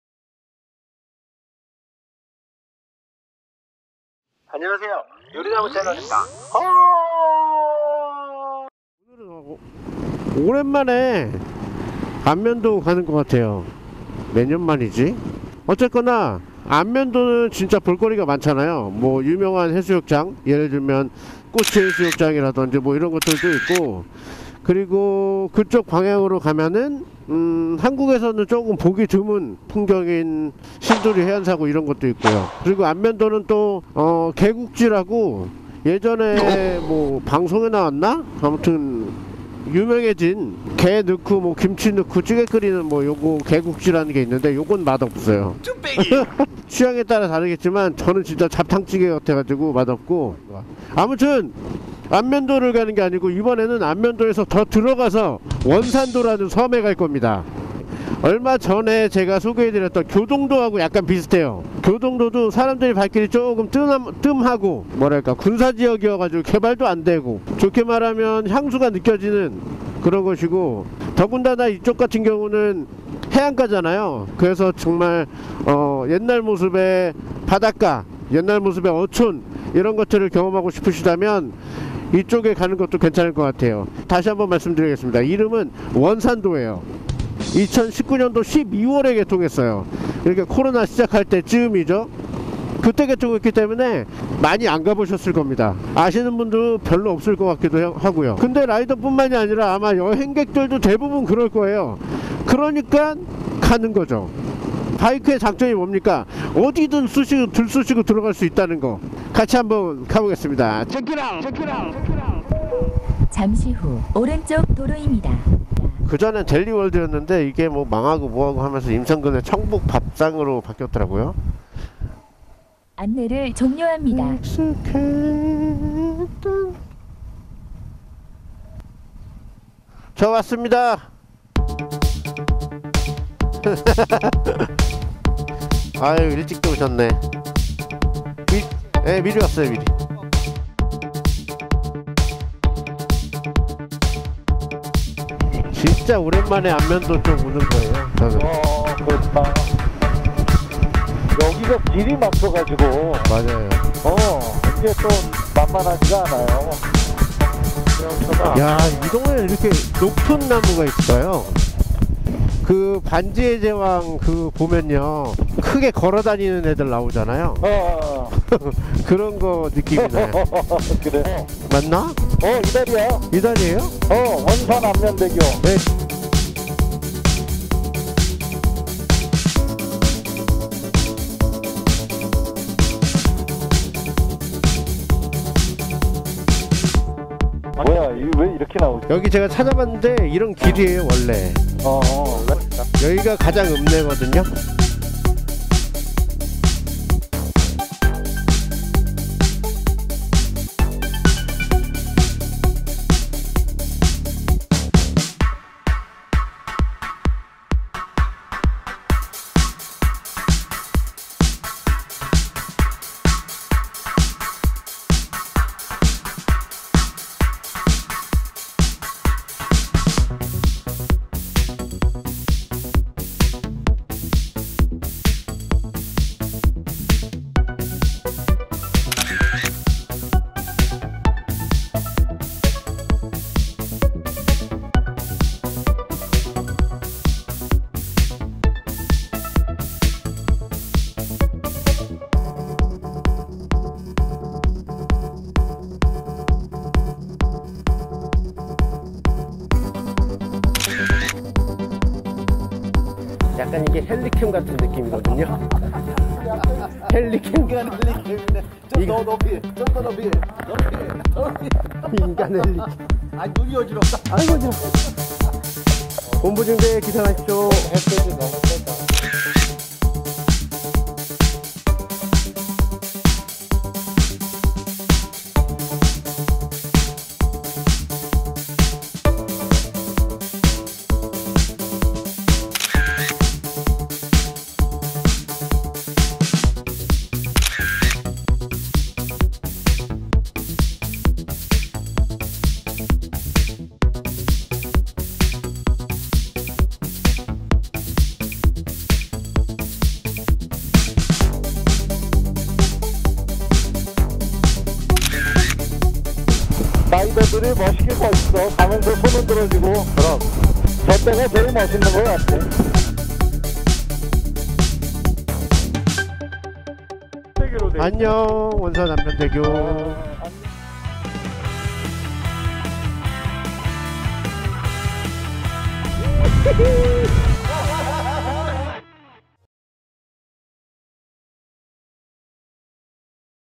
발길이 닿지 않은 어촌마을 원산도 여행
2019년 12월에 완공된 원산안면대교 를 통해
원산도 를 다녀왔습니다.
완공된지 1년밖에 되지 않아서 어촌마을을 구경할 수 있고요
편의점 같은건 없습니다. ㅋㅋㅋ
그래도 분위기 있는 카페 원산도리커피 도 가서 여유를 즐기고
잘 돌아왔습니다.

여러분도 드라이브, 여행, 캠핑 계획하고 있다면 많이 가보지 못한 섬으로
여행 어떠세요?